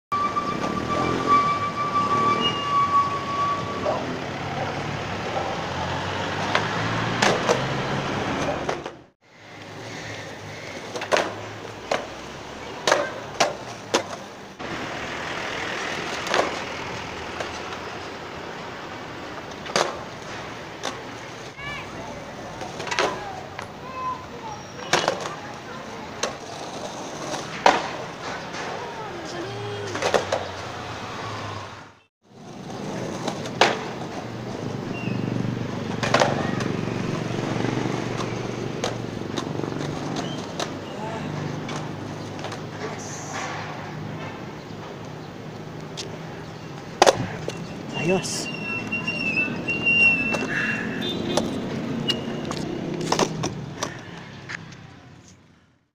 BLUNT STALL & NOSE MANUAL Cruiser Skateboarding Vincent Victor 49 Tuesday 060920 Manila
Published: June 13,2020 Saturday 2:19am

Category: Entertainment Skateboarding

Filmed & edited by myself Vincent Victor 49